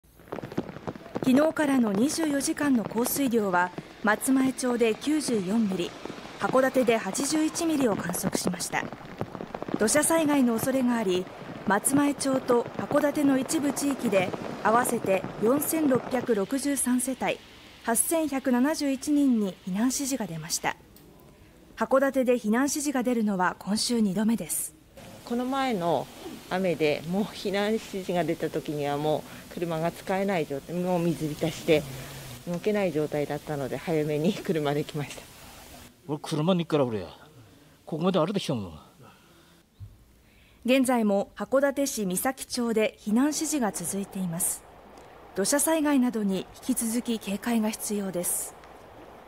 北海道南部 またも大雨 函館市は"今週2度目"の「避難指示」…土砂災害などに引き続き警戒を (22/08/12 11:55)
8月11日夜から12日朝にかけて北海道南部を中心に大雨となり、松前町や函館市に一時「避難指示」が出ました。

　11日からの24時間降水量は松前町で94ミリ、函館市で81ミリを観測しました。

　土砂災害の恐れがあり、松前町と函館市の一部地域であわせて4663世帯、8171人に避難指示が出ました。

　函館市で避難指示が出るのは今週2度目です。

　避難した市民：「この前の雨で避難指示が出たときには、車が水浸しで動けない状態だったので使えなかった。（今回は）早めに車で来ました」

　避難した市民：「車ないからここまで歩いてきた」

　12日午前11時30分現在も函館市御崎町で避難指示が続いています。

　土砂災害などに引き続き警戒が必要です。